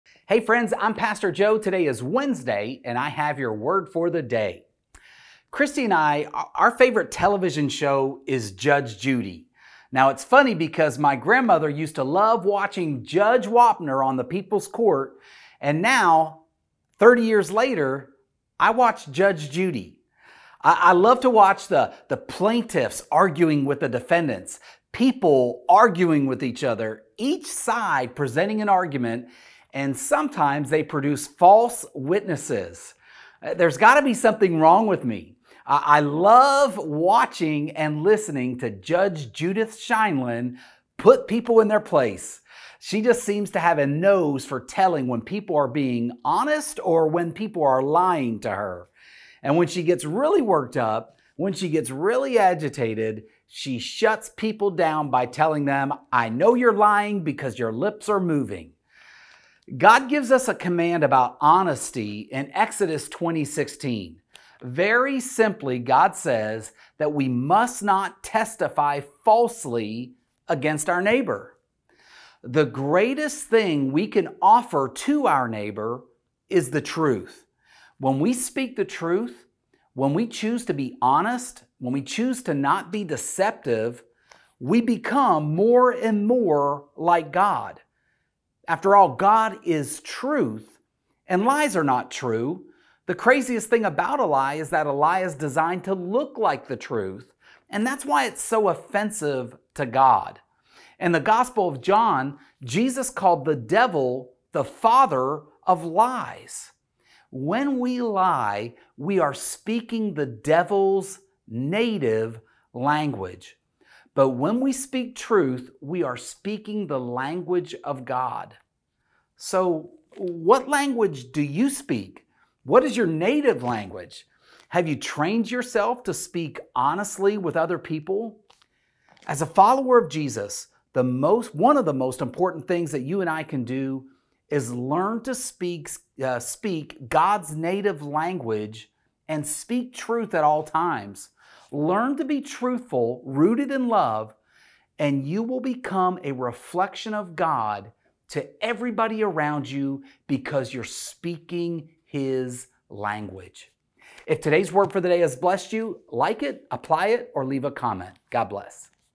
Your Word for the Day - Episode 532
Today Pastor Joe explains why he watches Judge Judy and how it correlates to the Bible.